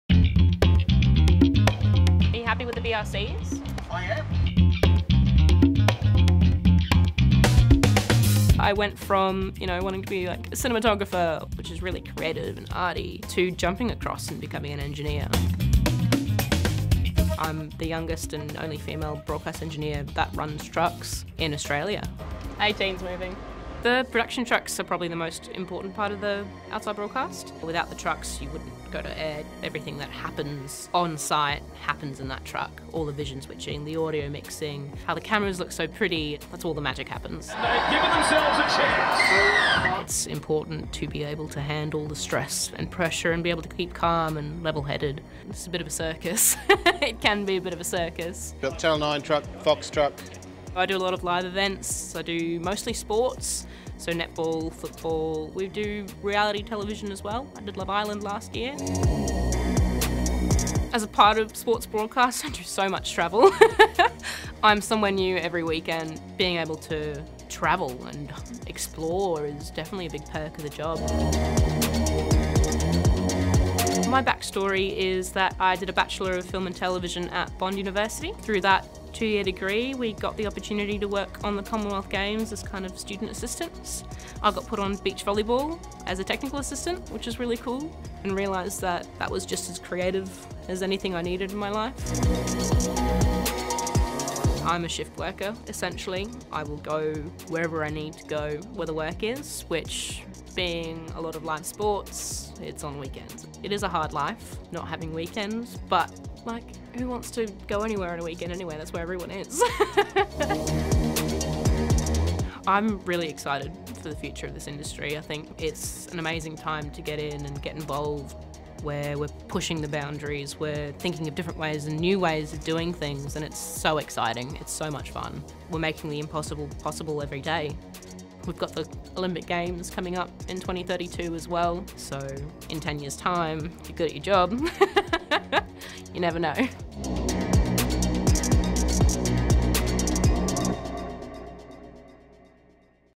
Meet Broadcast Engineer, Celine Wienand
Learn more about what it means to be a Broadcast Engineer at NEP Australia.

Have you ever considered a a career in broadcast technology?